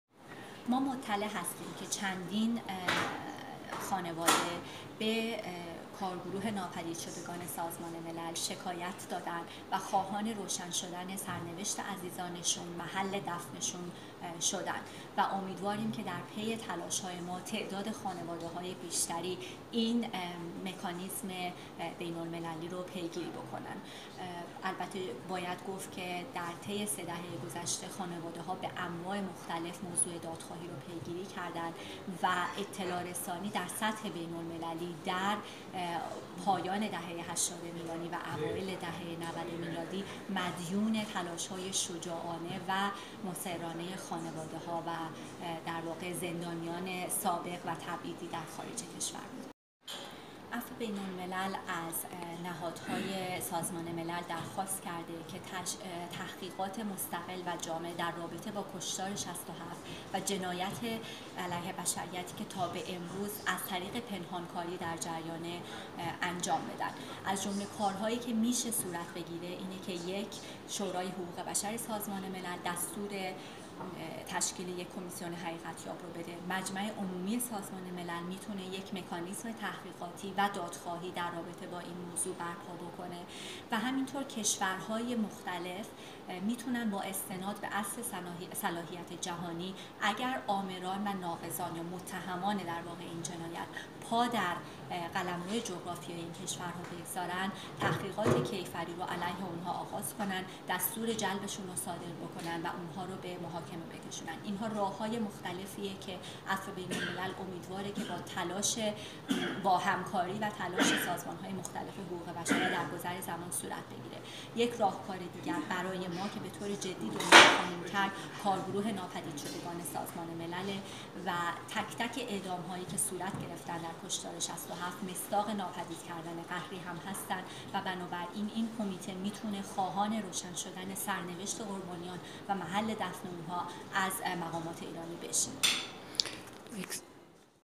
گفت‌وگو با رها بحرینی به مناسبت انتشار گزارش تازه سازمان عفو بین‌الملل
رها بحرینی، پژوهشگر سازمان عفو بین‌الملل در امور ایران، در گفت‌و‌گویی با زمانه گفت که عفو بین‌الملل اطلاع دارد که چندیدن خانواده به کارگروه ناپدیدشگان سازمان ملل شکایت و درخواست روشن شدن سرنوشت و محل دفن اعضا ناپدید شده خانواده خود را مطرح کرده‌د. به گفته رها بحرینی عفو بین‌الملل در تلاش است تا خانواده‌های بیشتری این مکانیزم بین‌المللی را دنبال کنند.

به گفته رها بحرینی، عفو بین‌الملل از نهادهای سازمان ملل درخواست کرده که تحقیقات مستقل و جامع درباره کشتار ۶۷ و جنایت علیه بشریت، که تا به امروز از طریق پنهانکاری در جریان است، را انجام بدهند.
رها بحرینی، پژوهشگر سازمان عفو بین‌الملل در گفت‌و‌گو با زمانه تأیید کرد که این سازمان برای نخستین بار در گزارش جدیدش دولت ایران را به «جنایت علیه بشریت» محکوم کرده است؛ در واقع اولین باری که یک نهاد بین‌المللی معتبر این اتهام را به جمهوری اسلامی وارد می‌کند.
به علاوه، اتهام وارد شده به دولت ایران تنها مربوط به گذشته نیست و عفو بین‌الملل جمهوری اسلامی را به جنایت «ادامه‌دار» علیه بشریت محکوم کرده است.